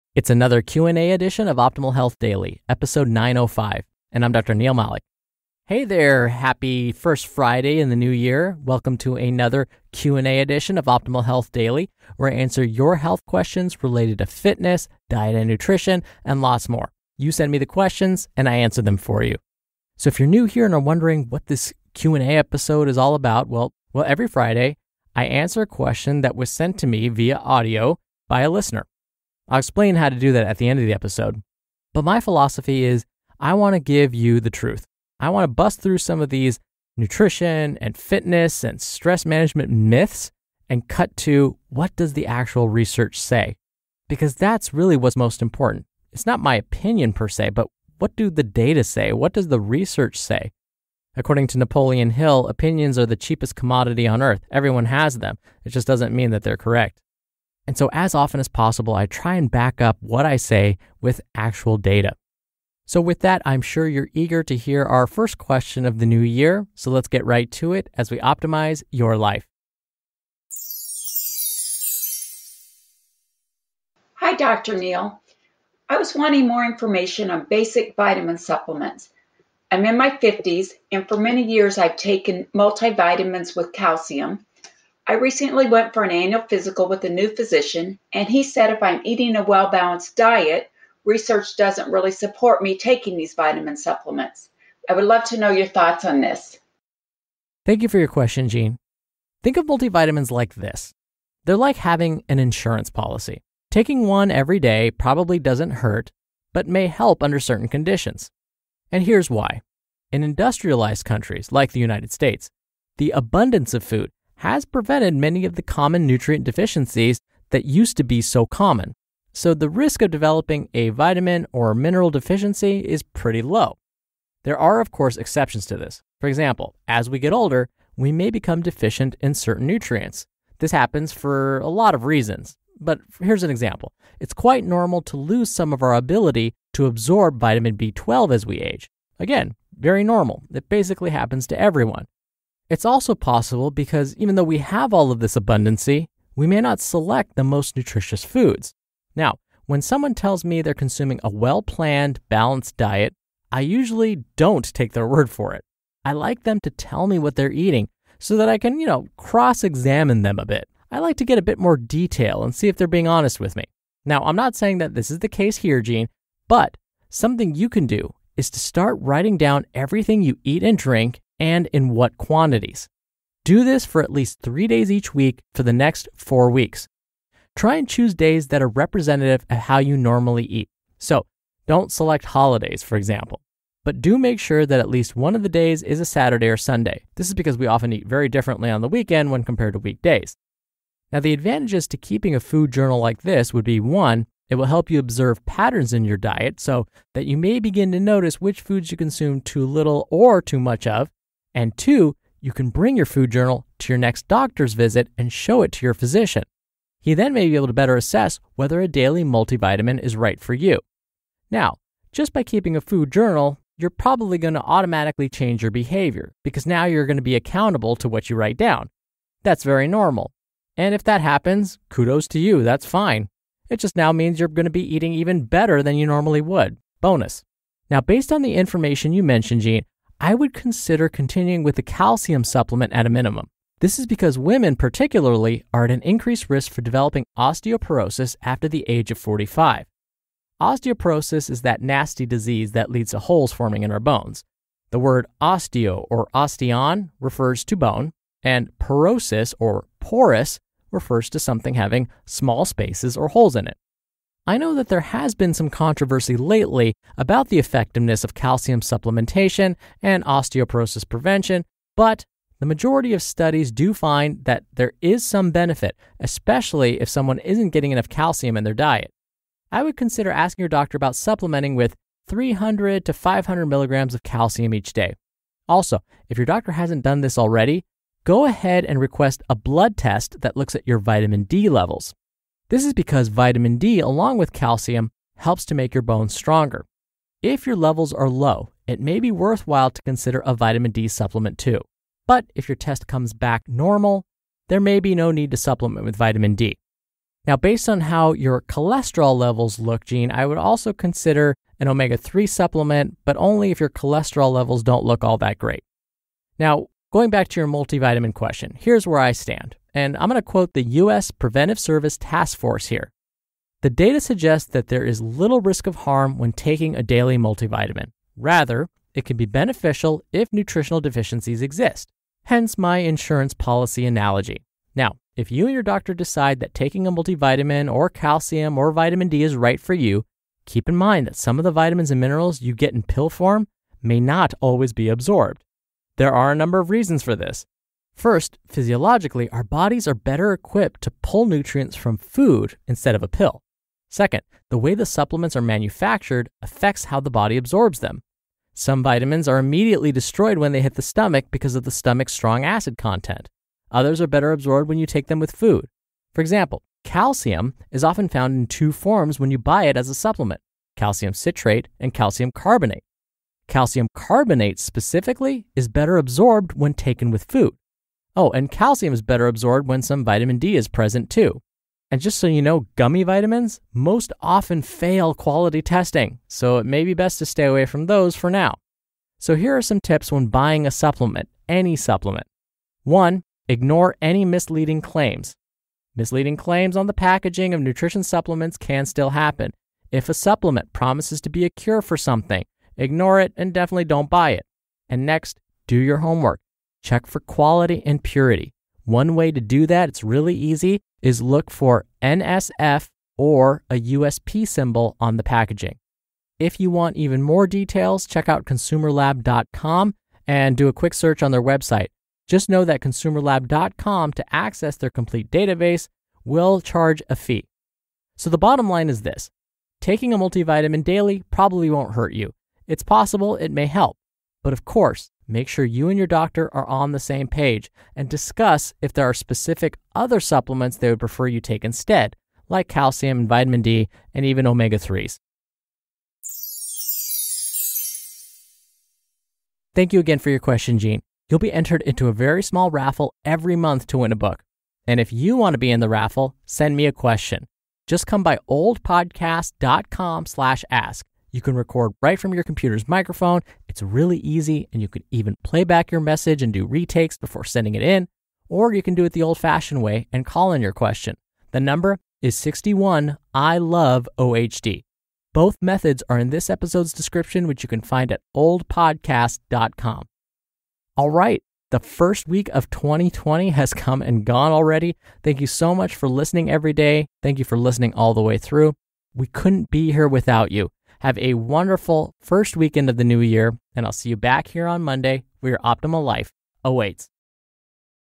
905: Q&A - Are Multivitamins Worth It - What Does Science Say About Taking a Vitamin Every Day
Episode 905: Q&amp;A - Are Multivitamins Worth It - What Does Science Say About Taking a Vitamin Every Day